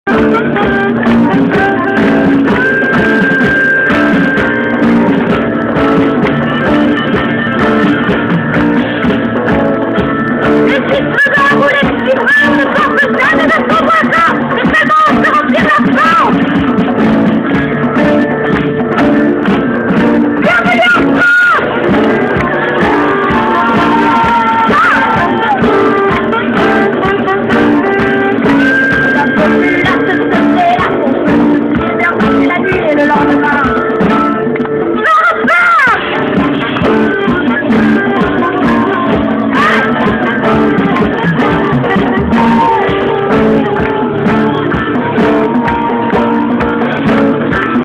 lou bramafam
lou bramafam groupe de ska au printemps des lycées 2007